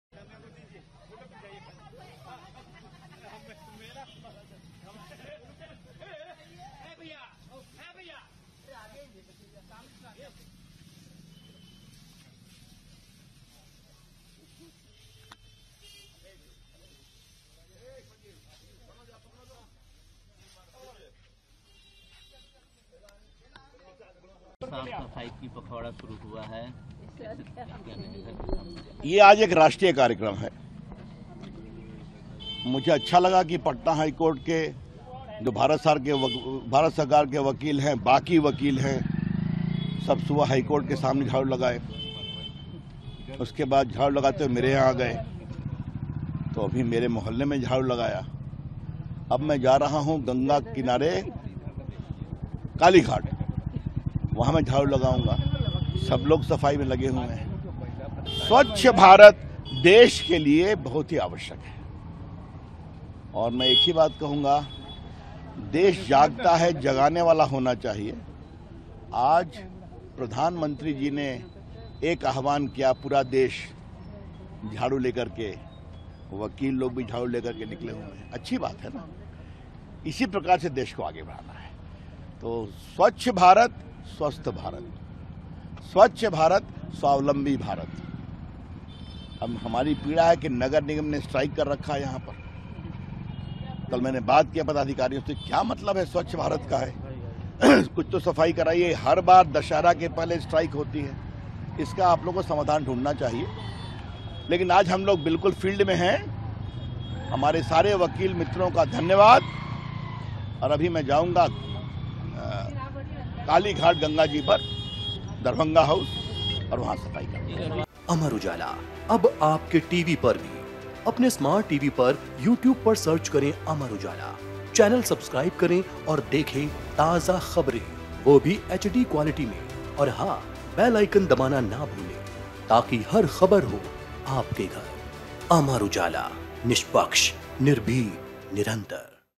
BJP नेता Ravi Shankar Prasad ने लिया 'Swachhta Abhiyan' में भाग, बोले- ये एक राष्ट्रीय कार्यक्रम है
रवि शंकर प्रसाद ने 'स्वच्छता अभियान' में भाग लिया. बोले- ये एक राष्ट्रीय कार्यक्रम है. स्वच्छ भारत देश के लिए बहुत आवश्यक होना चाहिए.